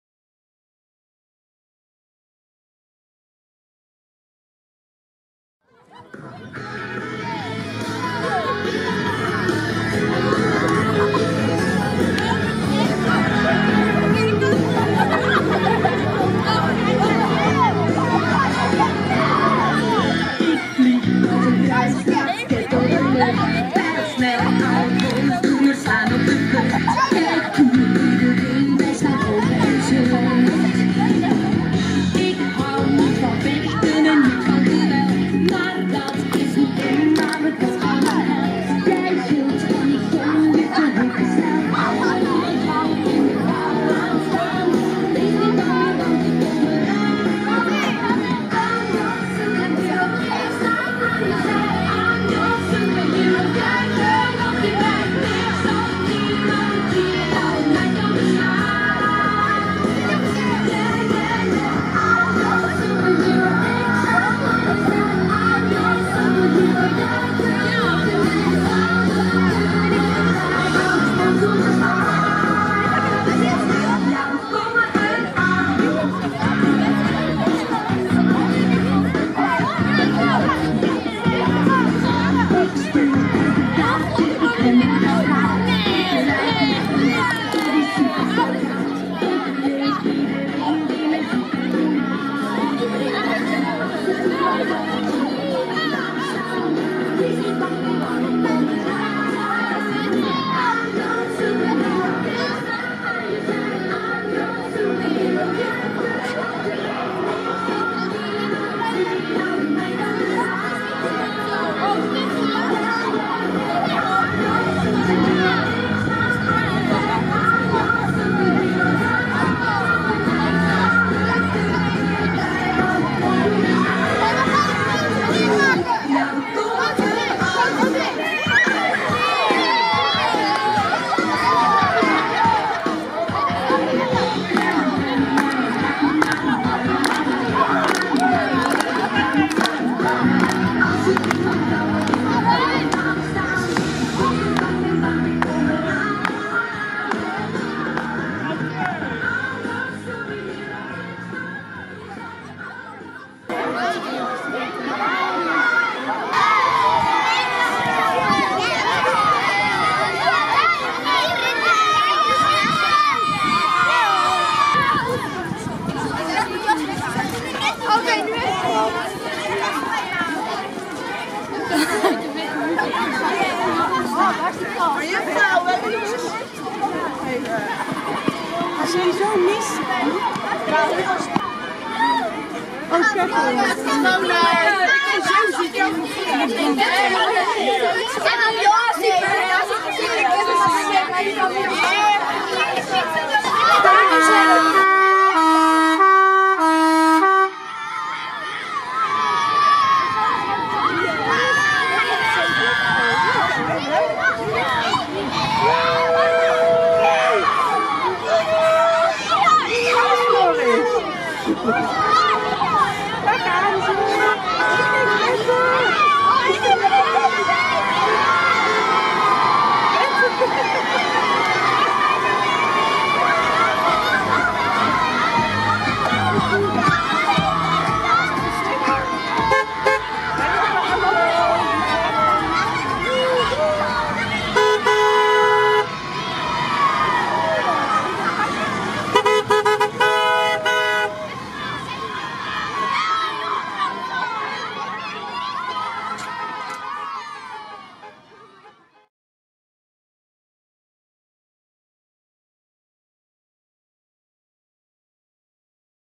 Uitzwaaien groep 8 schoolkamp Johannesschool 2016
Groep 8 van de Johannesschool uit Pijnacker is op kamp in Oosterhout geweest. Een kort video verslag van het uitzwaaien.